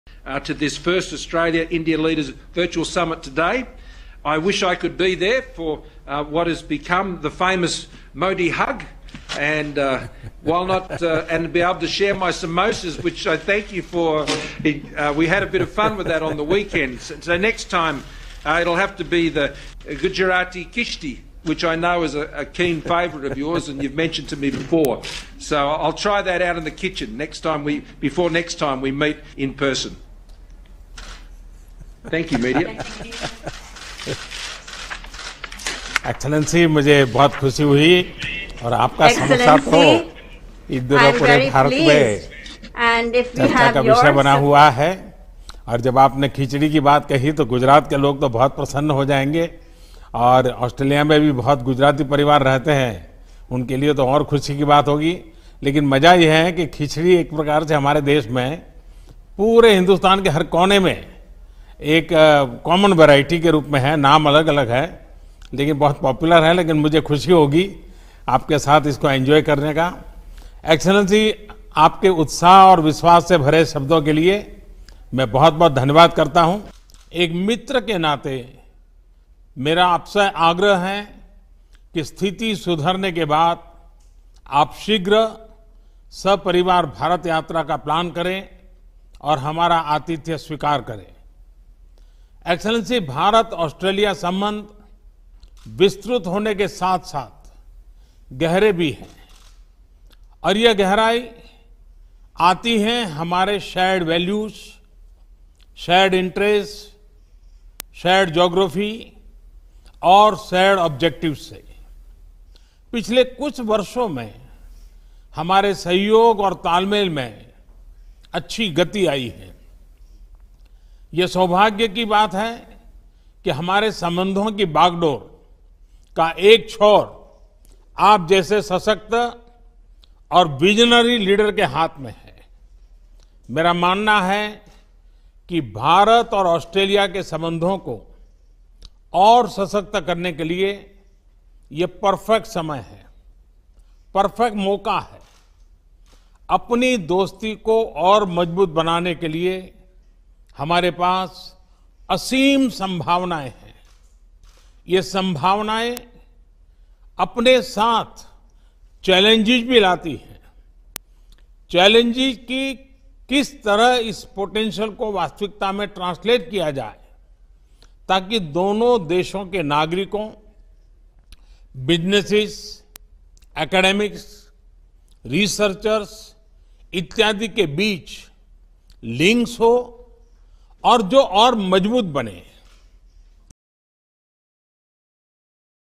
Bilateral Virtual Summit: खिचड़ी पकाएंगे Australian PM Scott Morrison, लगना चाहते हैं PM Modi के गले
भारत और ऑस्ट्रेलिया के प्रधानमंत्री नरेंद्र मोदी और स्कॉट मॉरिसन के बीच गुरुवार को वर्चुअल समिट में दोनों देशों ने दोस्ती को और मजबूत बनाने की जरूरत पर बल दिया। इस दौरान ऑस्ट्रेलिया के पीएम ने एक तरफ पीएम मोदी के गले लगने की इच्छा जताई तो दूसरी तरफ गुजराती खिचड़ी बनाने की बात भी कही।